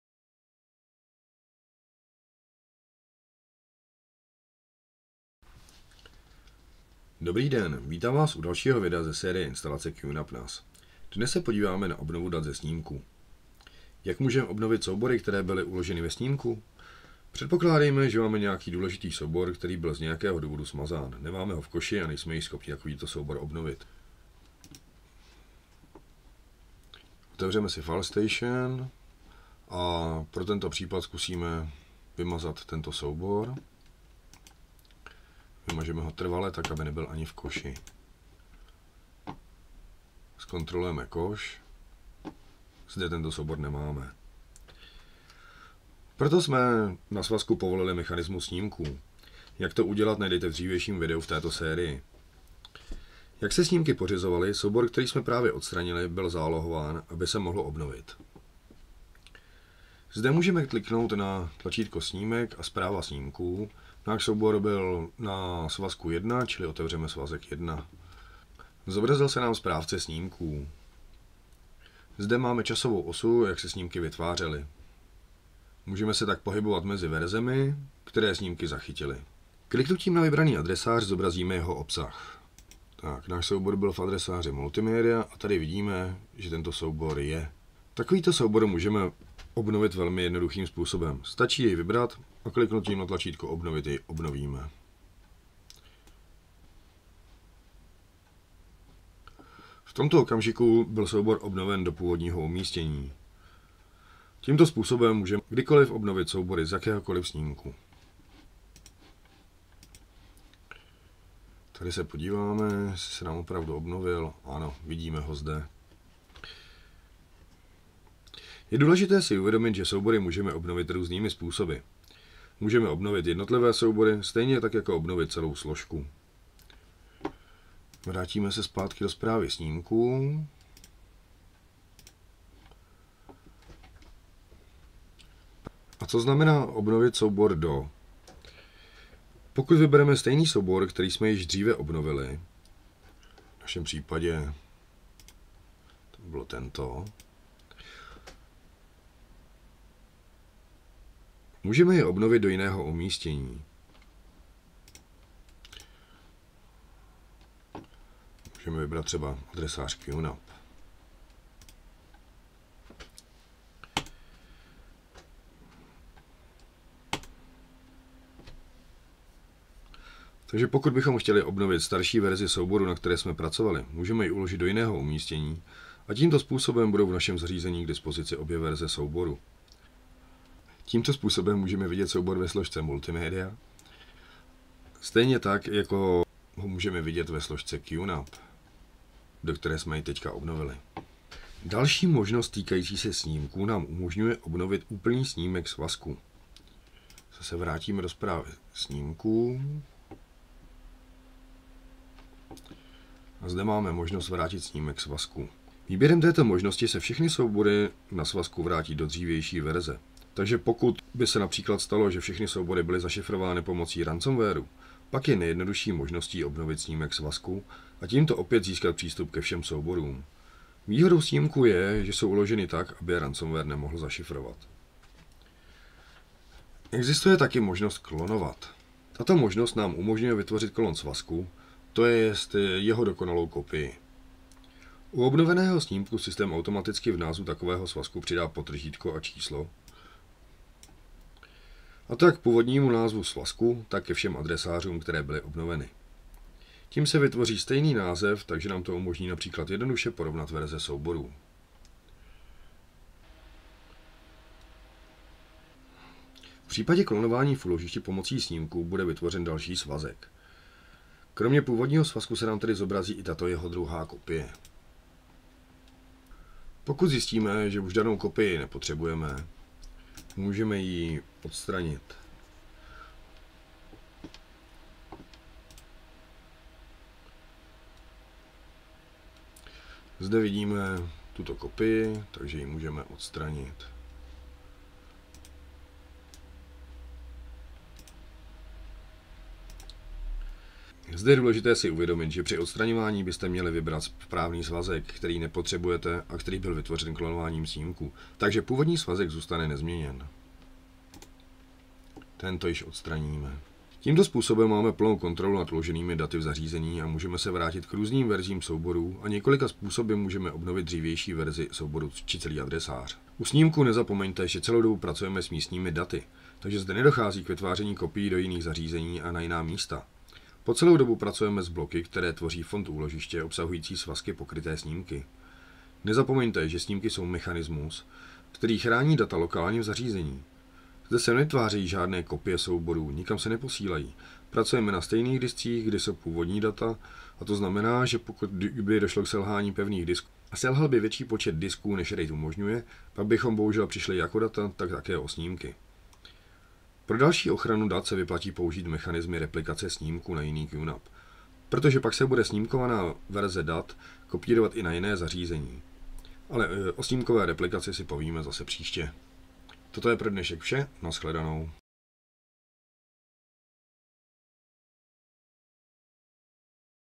Obnova dat ze snímků v QTS 5.0 na QNAP NAS
Video průvodce instalací operačního systému QTS 5.0 - Obnova dat ze snímků